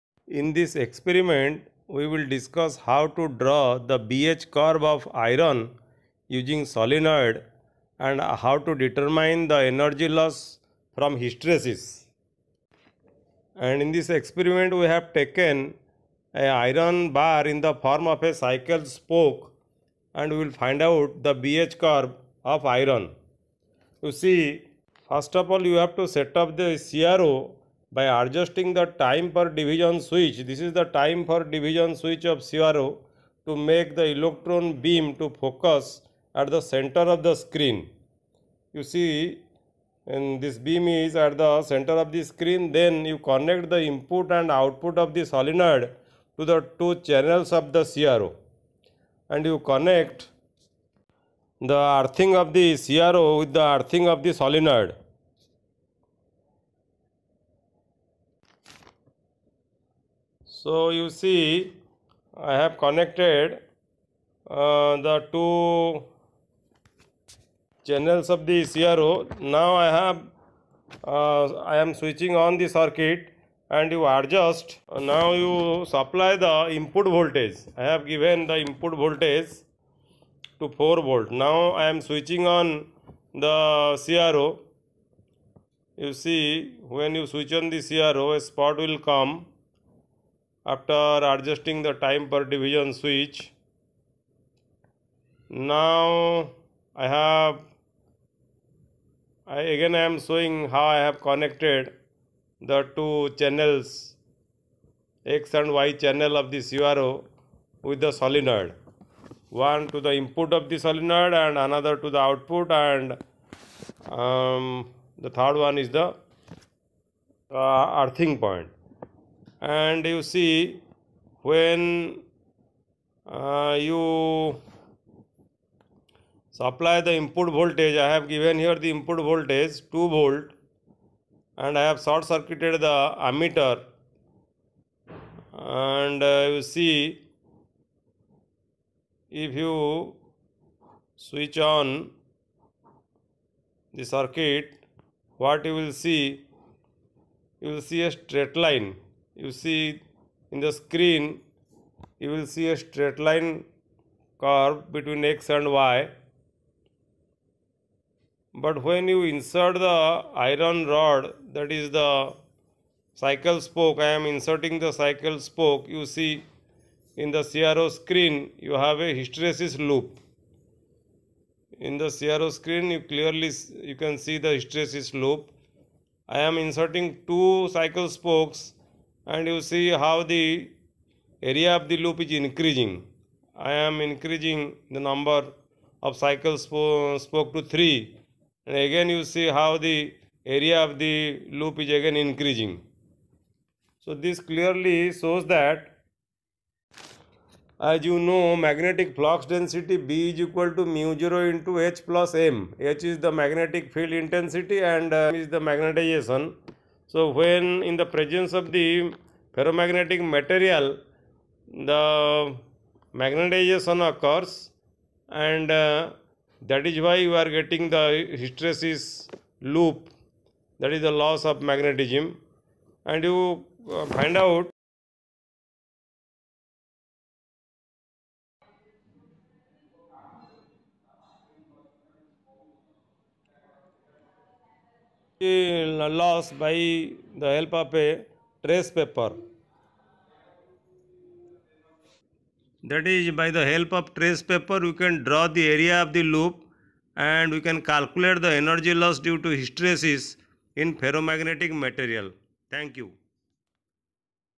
To draw the BH curve of Fe using solenoid and to determine the energy loss from hysteresis.
BH curve of iron experiment @PTE
hysteresis loss experiment @PTE